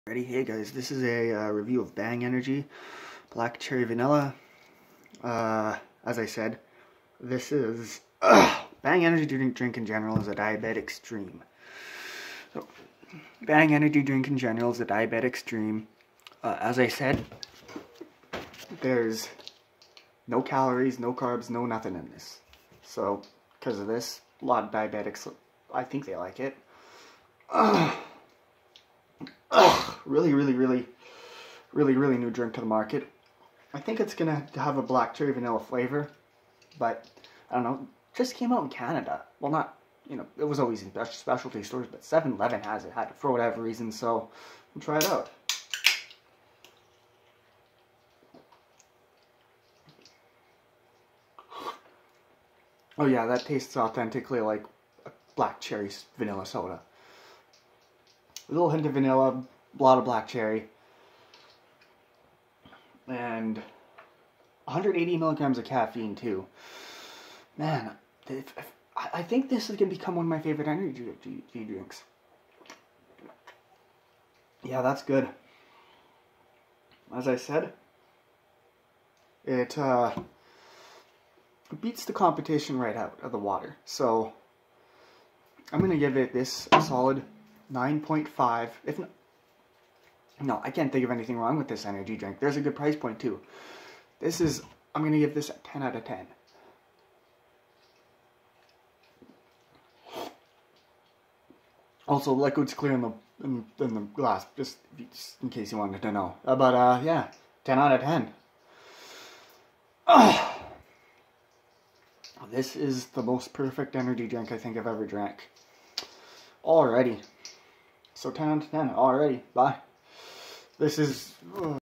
Bang Energy Black Cherry Vanilla review
A godsend for people on a keto diet and diabetics, Bang Energy Drink has no calories or carbs, and actually tastes like a black Cherry soda. I was very impressed by this drink and plan to make it my main energy drink of choice